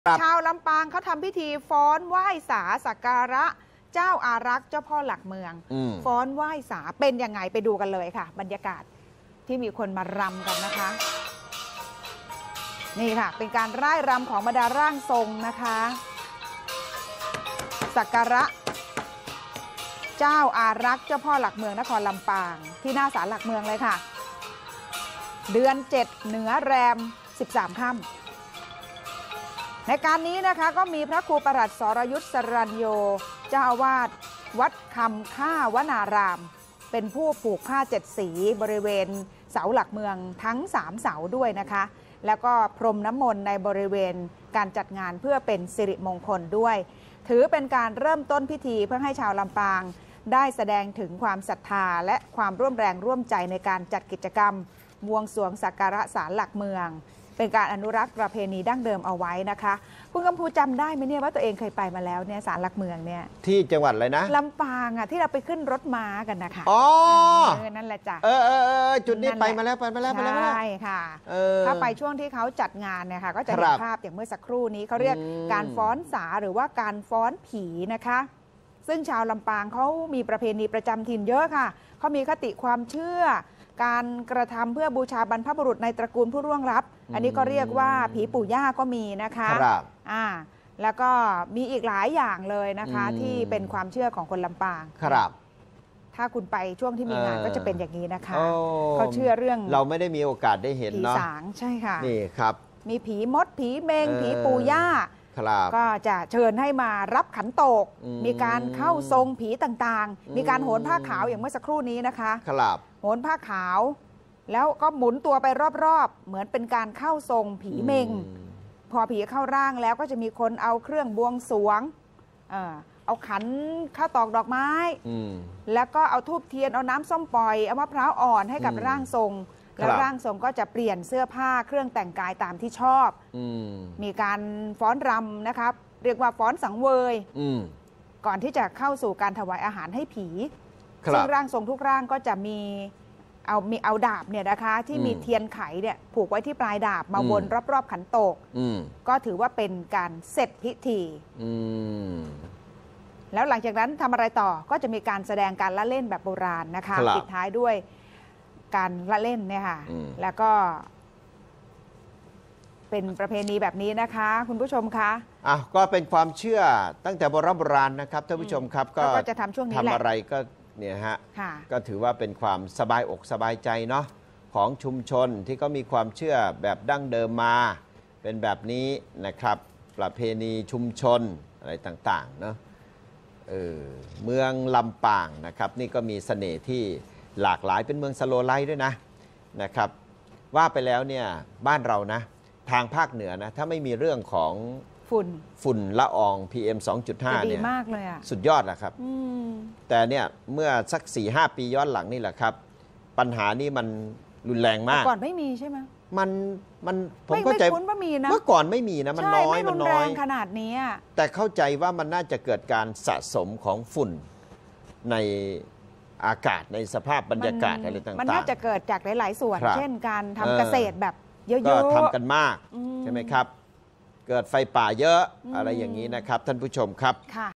หาชมยาก ฟ้อนผีไหว้สาสักการะพ่อเจ้าหลักเมืองลำปาง
ลำปาง 23 พ.ค.-ชาวลำปางร่วมพิธีฟ้อนไหว้สา สักการะเจ้าอารักษ์พ่อเจ้าหลักเมือง เพื่อความเป็นศิริมงคล อนุรักษ์ประเพณีเก่าแก่ของชาวล้านนา โดยมีร่างทรงเข้าร่วมนับ 100 คน